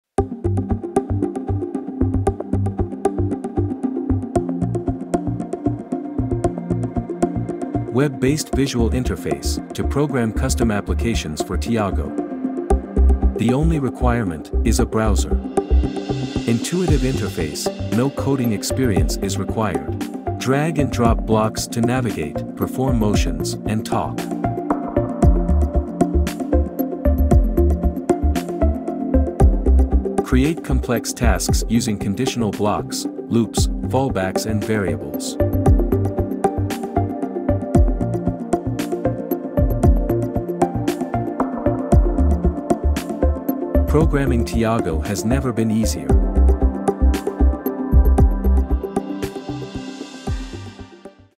PAL Robotics | TIAGo - Visual Programming Package
This video provides a comprehensive demonstration of the Visual Programming software package's capabilities and how to effectively use it. Discover how to effortlessly program your TIAGo robot using nothing but your web browser. This package is designed to be user-friendly and requires no prior programming experience, thanks to its intuitive interface and building block approach. Create and launch programs for TIAGo without writing a single line of code!